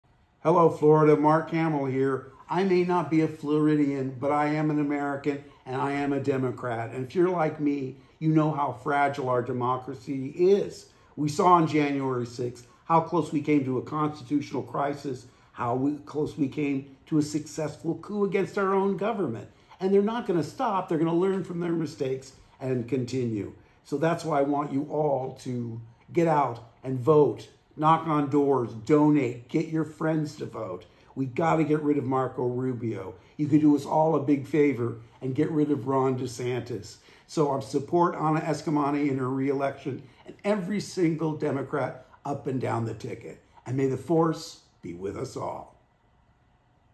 Mark Hamill Gives Florida Democrats a Boost
Forever grateful when Mark Hamill gives Florida Democrats a boost!! Thank you for sending us this awesome video to play during the Leadership Blue dinner & may the force be with us! 💪